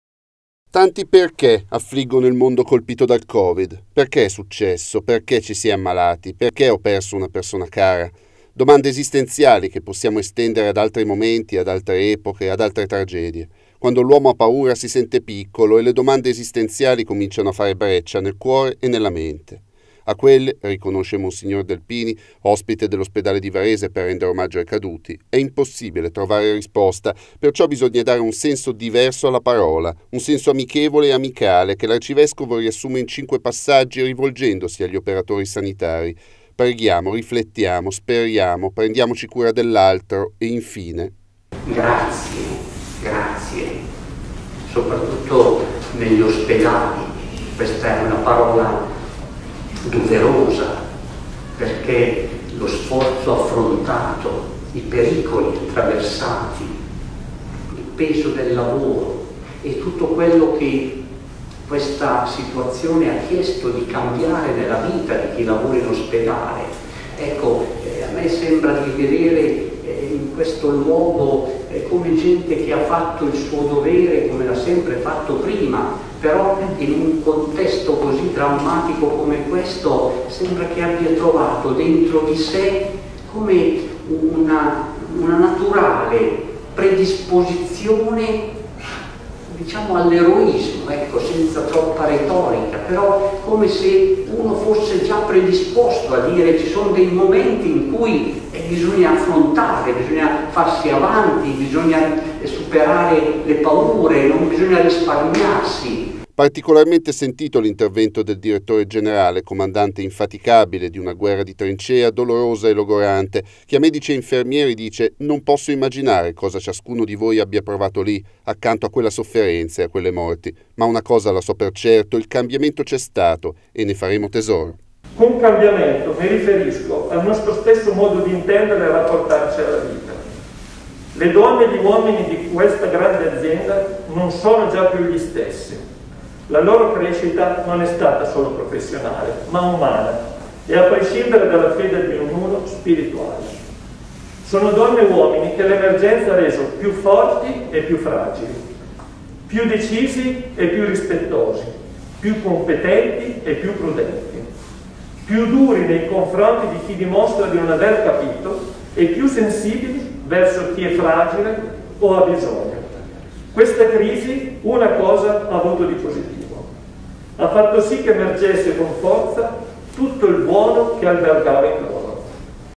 Covid, fede e speranza. "Nulla sarà più come prima"
La messa celebrata dall'Arcivescovo Delpini all'Ospedale di Circolo diventa un'occasione di profonda riflessione per chi, da oltre un anno, combatte in prima linea. Bonelli: "Tutto è cambiato, nella sanità e nella società".